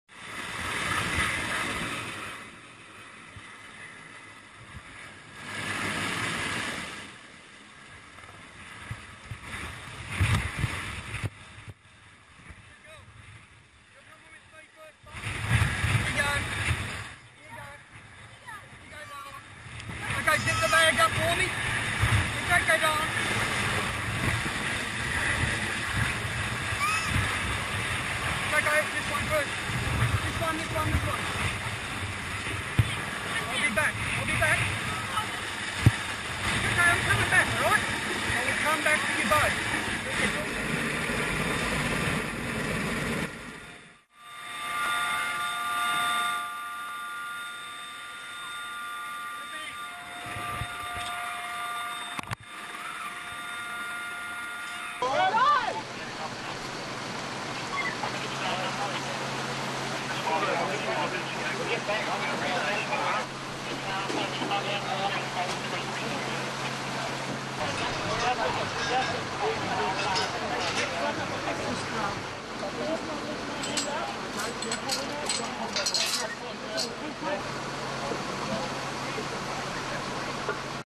BBC News - Australia floods: Toddler is winched to safety
A young child and two women have been airlifted to safety after their car became trapped in fast-rising flood waters in Bundaberg, in the Australian state of Queensland.

In a separate rescue in Hervey Bay, fire and rescue officers freed a man trapped under a drain cover.

Queensland is on alert for flooding in the wake of Tropical Cyclone Oswald.